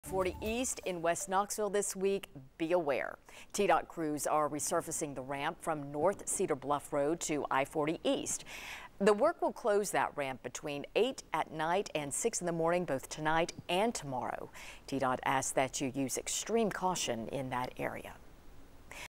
TDOT crews to close I-40 ramp for repaving
TDOT CREWS ARE RESURFACING THE RAMP FROM NORTH CEDAR BLUFF ROAD TO I-40 EAST.